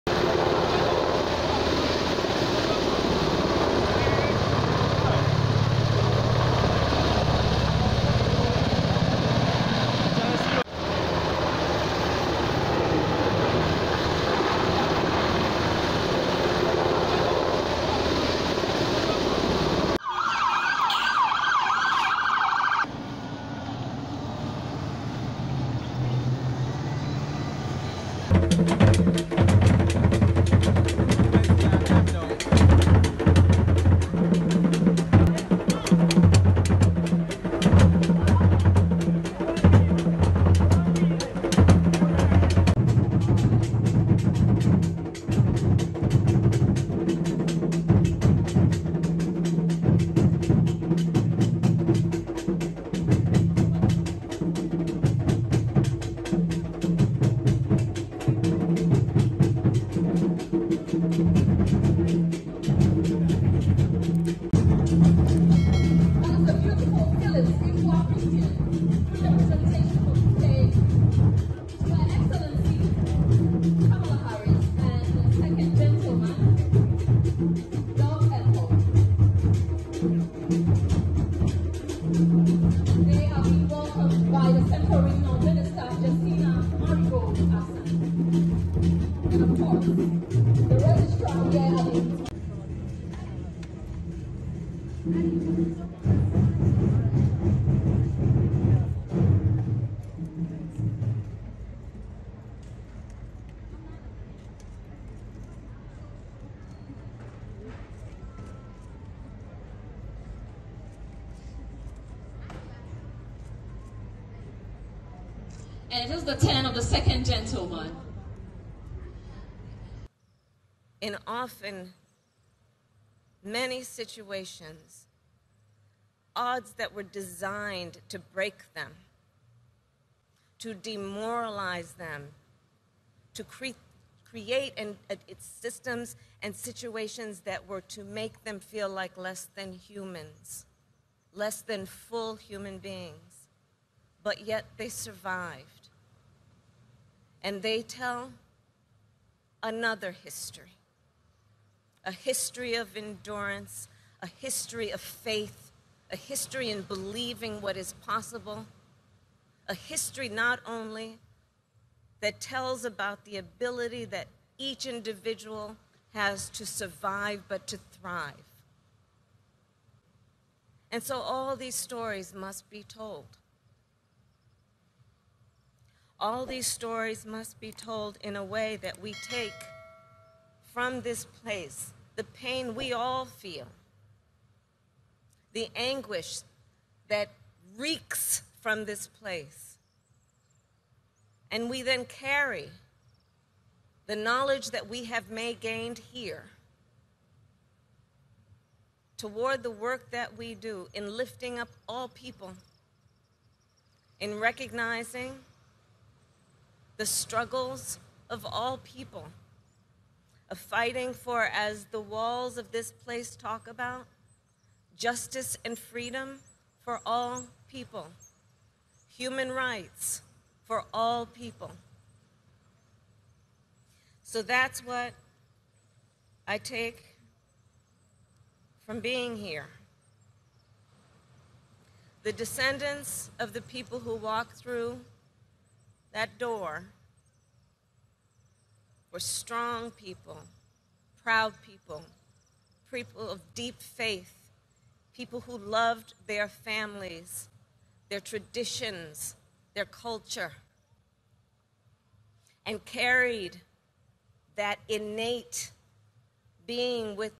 US Vice President KAMALA HARRIS Visit Cape Coast | Tours Cape Coast Castle
The United States of America's Vice President Kamala Harris and Second Gentlemen Doug Emhoff arrived in Ghana to commence a three-day state visit, becoming the first Black woman US vice president to visit Africa 🇺🇸🤝🏾🇬🇭
As part of her visit,, she visited the Emintsimadze Palace in Cape Coast and toured the Cape Coast Castle and Museum / Slave Dungeon.